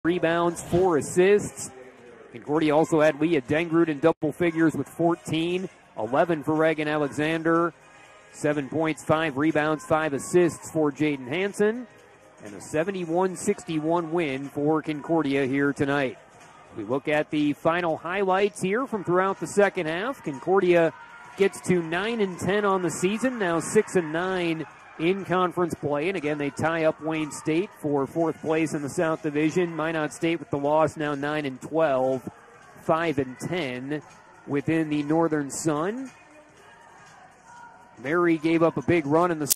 2022-23 Concordia-St. Paul women's basketball highlights vs Minot State, 1-27-23
Concordia-St. Paul tops Minot State in NSIC women's basketball at Gangelhoff Center. Audio commentary by Dan Flanagan, voice of the Golden Bears